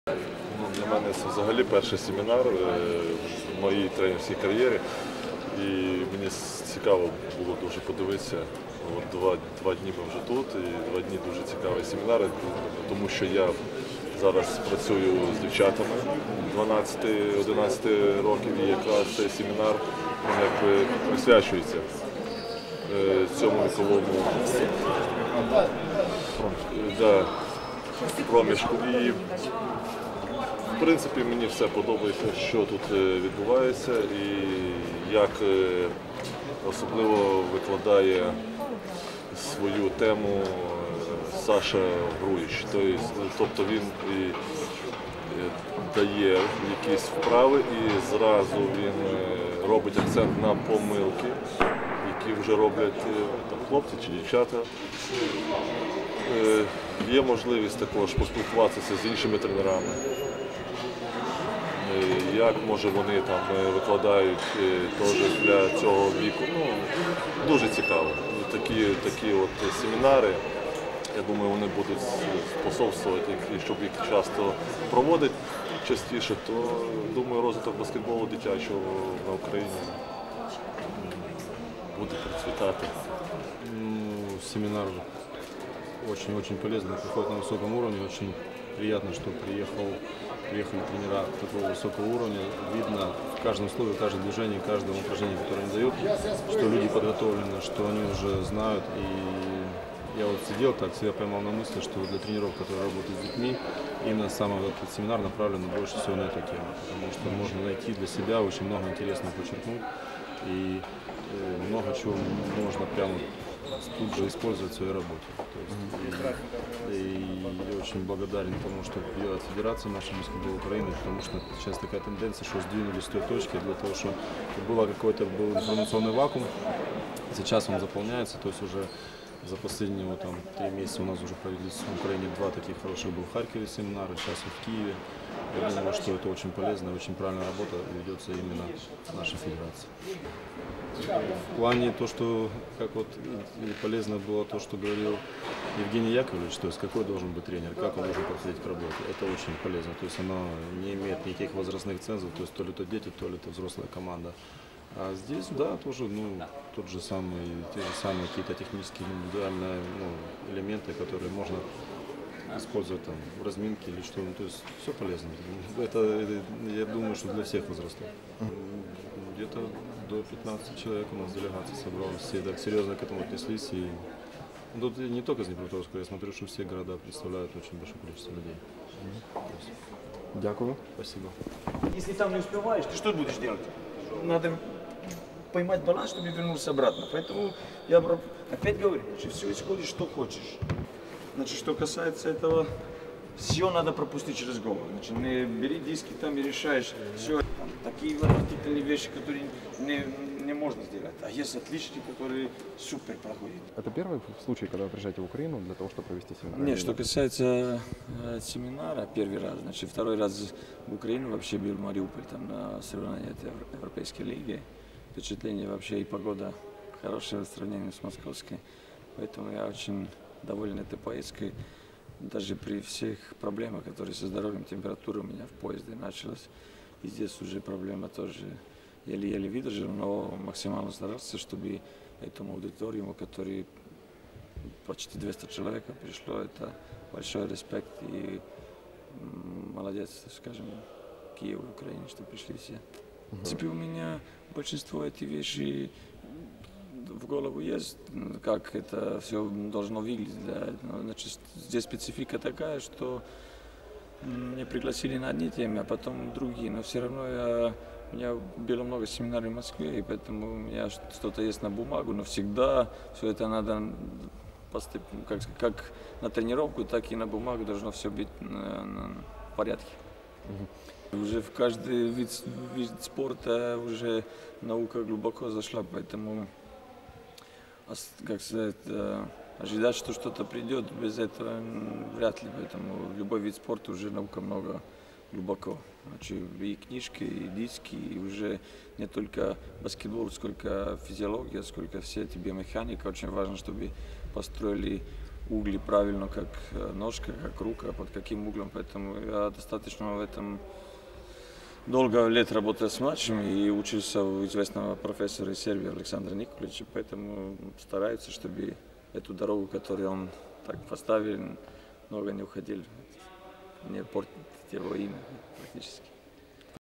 Міжнародний тренерський семінар у Києві. ІНТЕРВ'Ю
У Києві протягом трьох днів спілкувалися та обмінювалися досвідом більше 200 тренерів.

Інтерв'ю:

1. В'ячеслав Ковалевський — дитячий тренер (Рівне)
2. Дмитро Марков — старший тренер юніорської збірної України (U-18)
3. Саша Груїч — тренер БК «Динамо» (Москва)